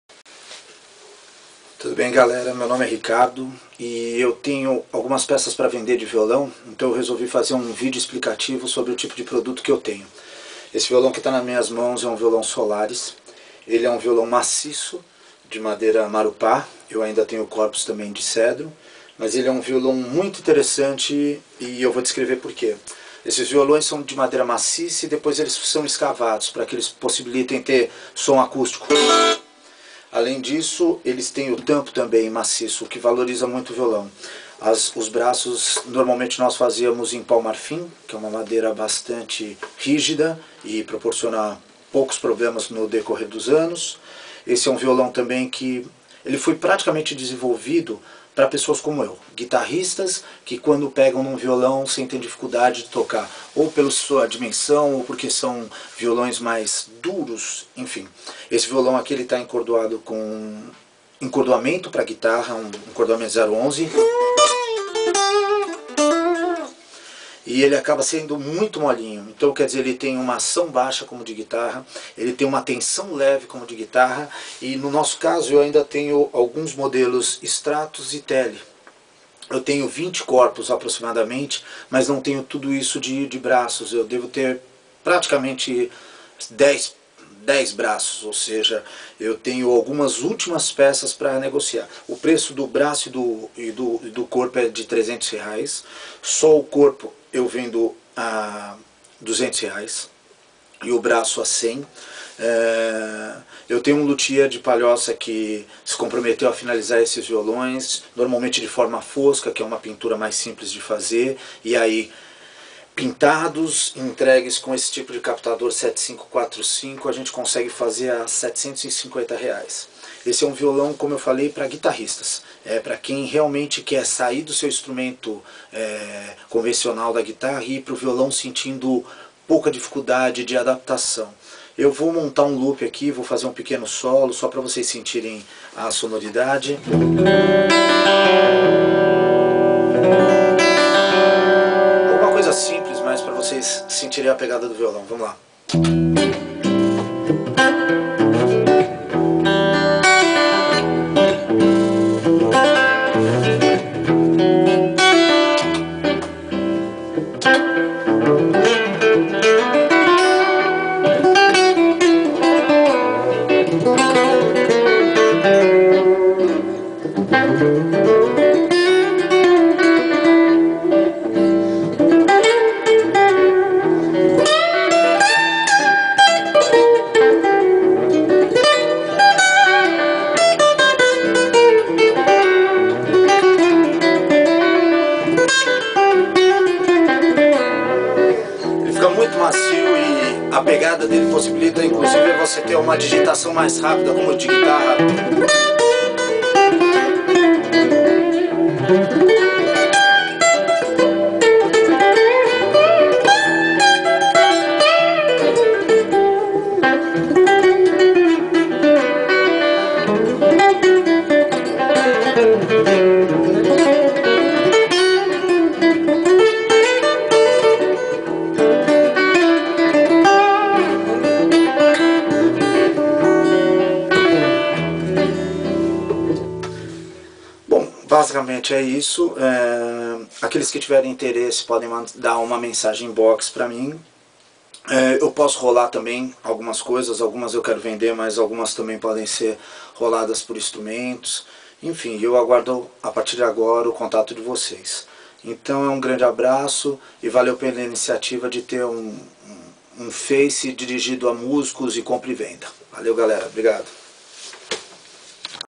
Video solarez guitars acustic
Video demonstrativo dos violões Solarez que tenho para vender... R$ 750,00 corpo e braço montados. Corpos e braços crus a R$ 300,00.